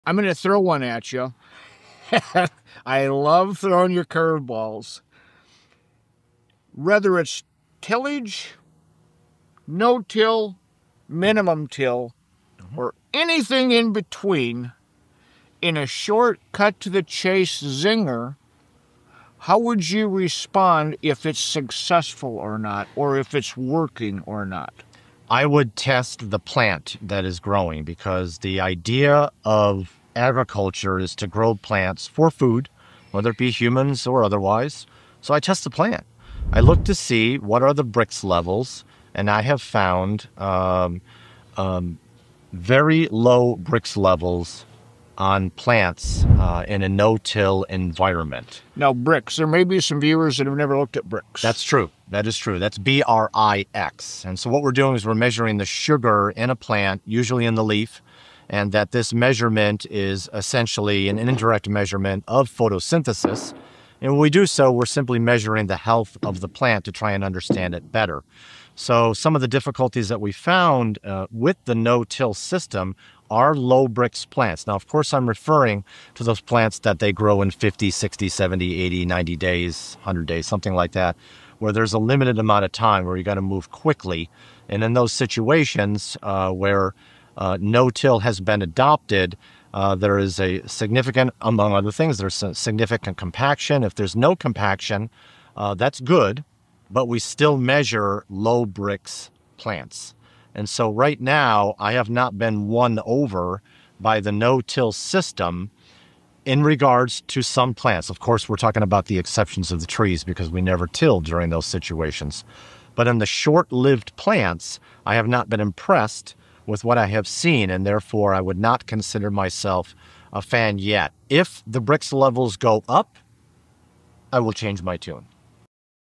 Does No-Till = Less Healthy, Low BRIX Crops?
Does no-till farming lead to less healthy, low Brix crops? Dr. Dykstra explains why measuring Brix levels - a key indicator of plant health - is crucial and why no-till might not be the best approach for short-lived crops. Watch to learn why compaction and low Brix go hand in hand. Could intelligent, strategic tillage (at the very least) be part of the answer? In the full episode, Glen also shares how GSR Calcium can help correct compaction issues in many systems, including no-till.